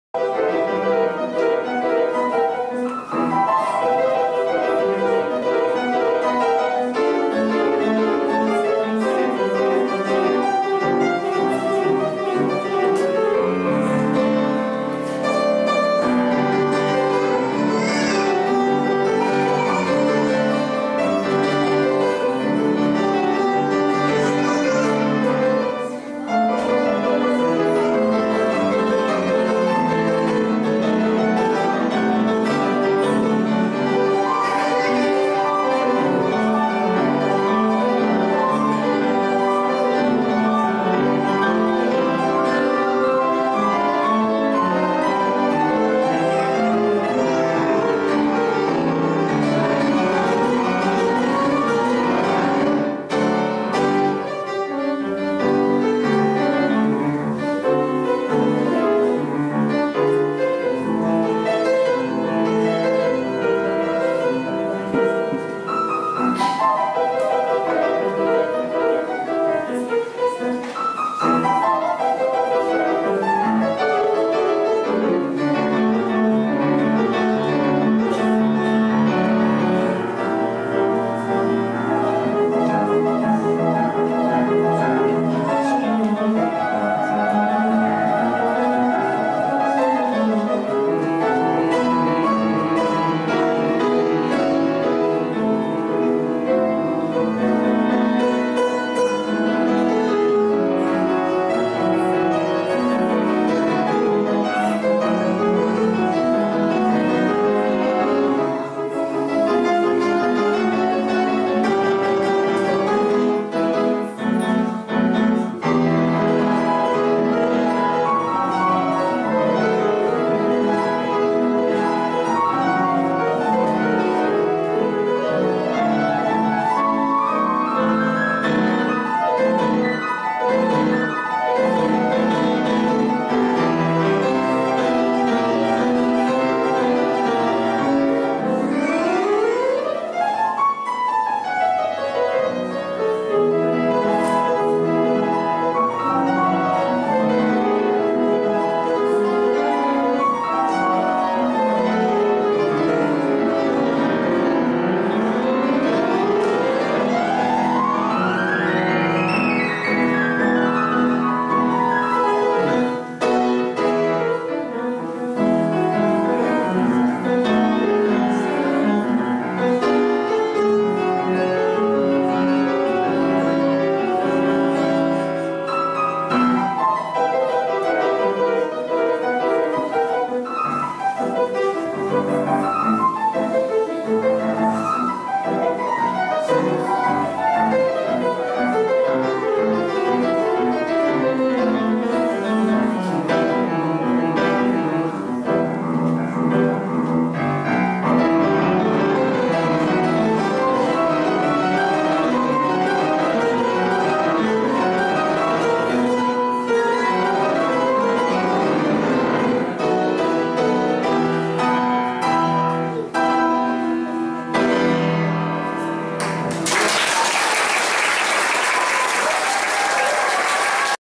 Nisan Dec 2017
An earlier video showcasing the talent of our accompanist, Nisan Tuazon